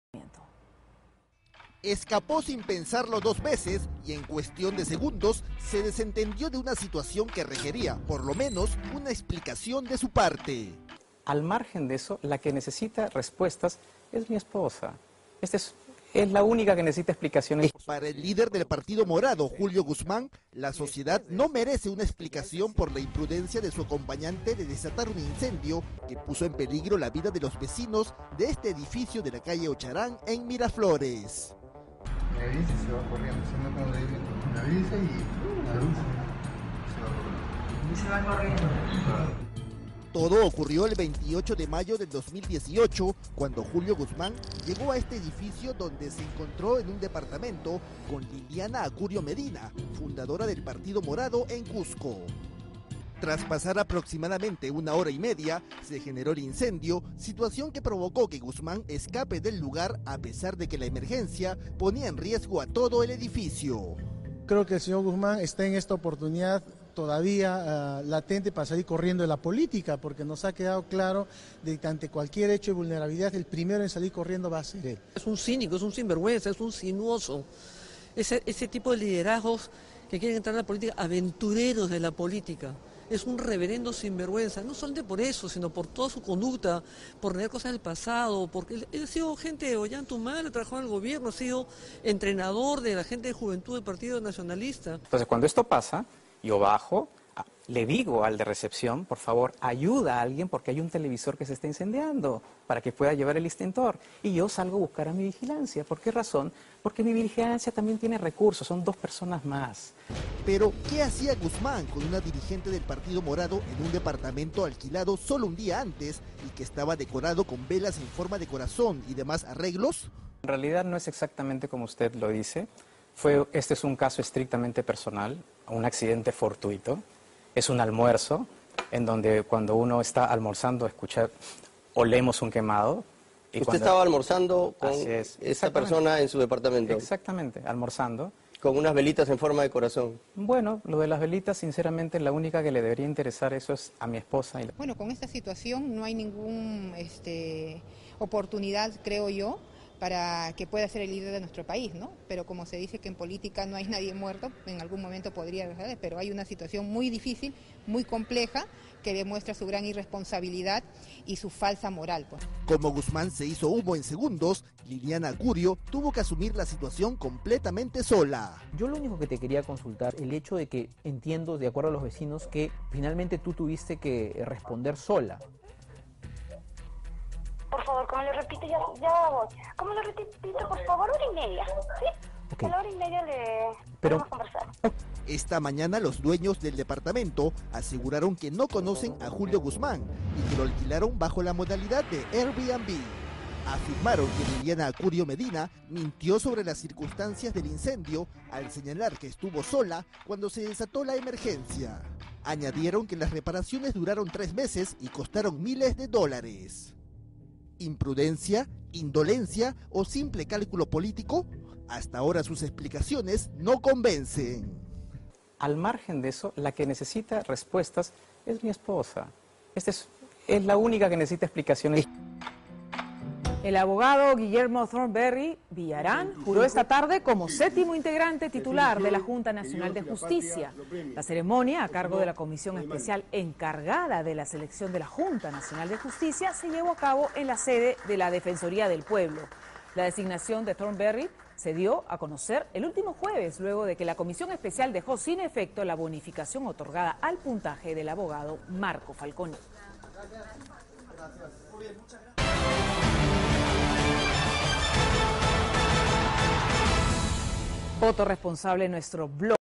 ¿Imprudencia? ¿Estrategia de marketing? Esto dijeron los excongresistas del caso Julio Guzmán
Tras la reciente investigación de Panorama que da cuenta de un accidente que involucra al líder del Partido Morado, Julio Guzmán, diversos excongresistas así como miembros de la Comisión Permanente criticaron duramente su actuar.

Aunque el caso recién fue difundido, ocurrió el 28 de mayo del año pasado cuando Guzmán se reunió para un presunto almuerzo con la fundadora de este partido en Cusco, Liliana Acurio. Este encuentro ocurrió en un departamento en Miraflores, donde hora y media más tarde se generó un gran incendio que provocó que Guzmán huya de escena.